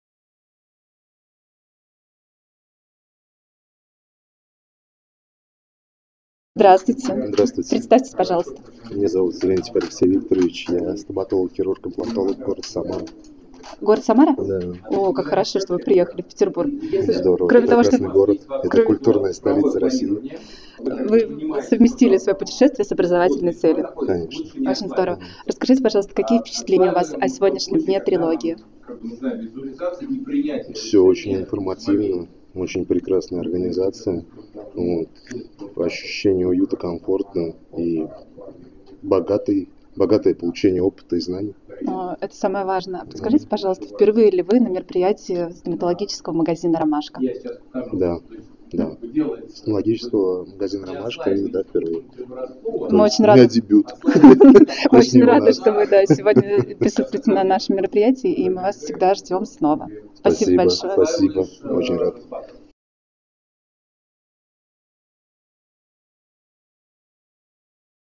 Алексей Терентьев. Хирургическая трилогия 2024. Отзыв №12
Практический семинар Александр Дмитриевич Лысов
Темы:" Пластика мягких тканей в периимплантной зоне", " Одномоментная имплантация с пластикой мягких тканей", " Индивидуальные заживляющие абатменты (ИЗА)"
30 марта - 4 апреля 2024
«Хирургическая трилогия» в Санкт-Петербурге!
3 звездных лектора: Виталий Панцулая, Александр Лысов, Мария Носова.
6 дней непрерывного погружения в продвинутую пластическую хирургию мягких тканей и костную пластику. Демонстрации, мастер-классы, практические занятия.